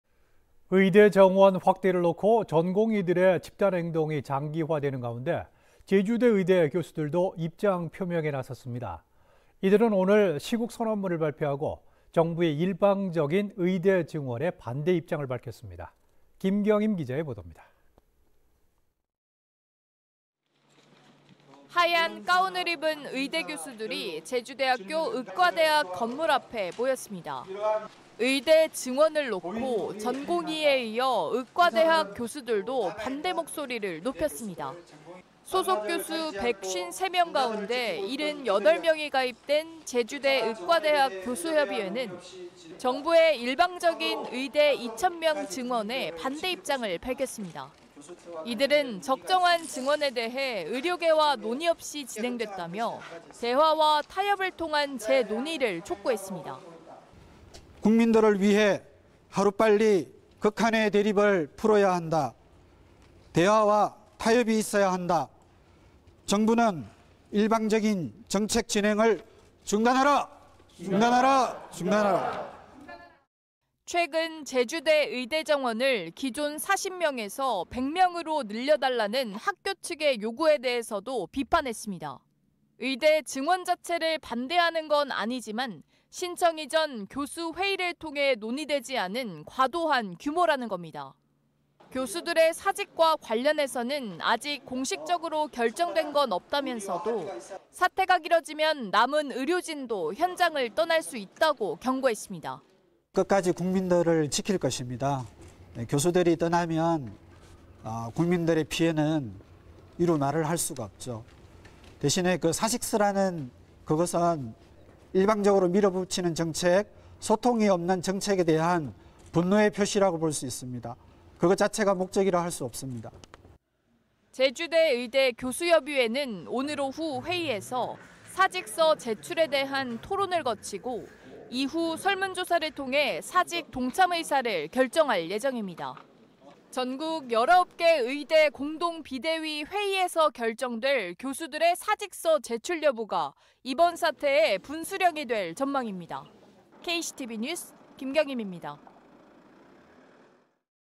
제주대 의대 교수들 "증원 반발"…사직서 낼까?
의대 정원 확대를 놓고 전공의 집단행동이 장기화되는 가운데 제주대 의대 교수들도 입장 표명에 나섰습니다.

이들은 오늘 시국선언문을 발표하고 정부의 일방적인 의대 증원에 반대 입장을 밝혔습니다.